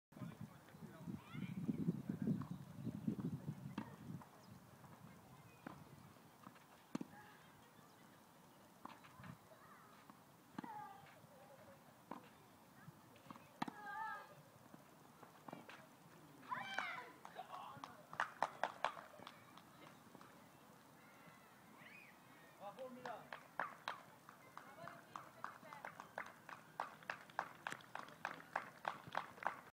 Milla Sequeira beating her 1st R5 at the age of 10
Milla Sequeira just beat her 1st R5 in the semis of a tournament, she's 10. This is the match point.